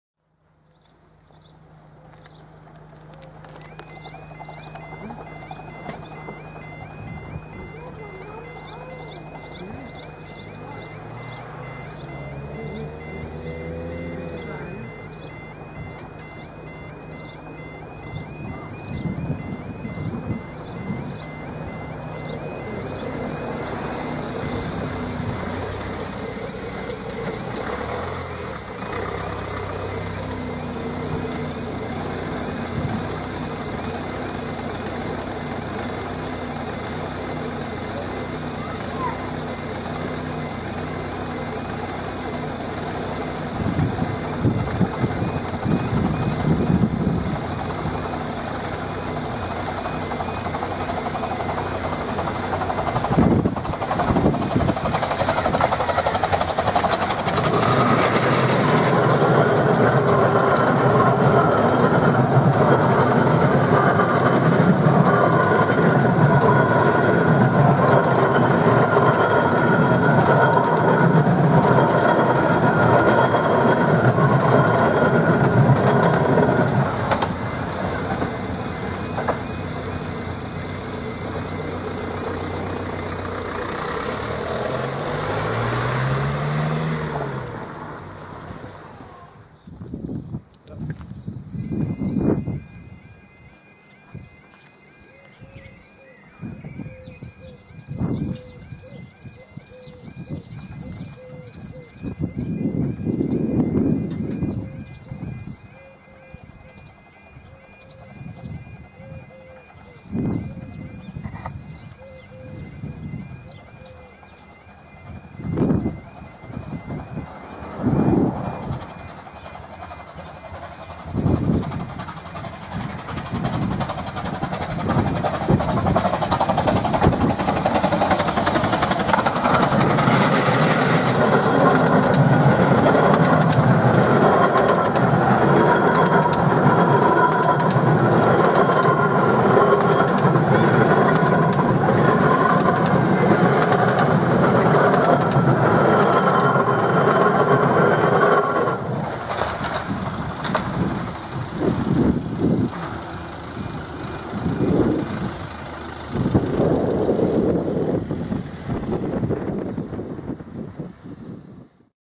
Lydd-on-Sea Level Crossing (Taylor Road Crossing)
Romney, Hythe & Dymchurch Railway steam loco No.2 "Northern Chief" in Brunswick Green passes the Taylor Road crossing by Coast Drive heading to Dungeness and Romney Sands respectively. This crossing is not too far from Hull Road crossing, and both lower together but raise separately. This is another crossing on the line with newly fitted barriers, and they seem to be as normal as they can be. The signs that have been patched by the council (as the crossing has changed from open to gated) are appalling, as they only cover part of the old sign and white is still visible on the massive sticker of the gate warning sign. A pub here was demolished, probably because the shop next to the crossing was more of a convenience.
Route: Romney, Hythe & Dymchurch Railway
County: Kent
Trains:
0:02 - 0:56
1:30 - 2:08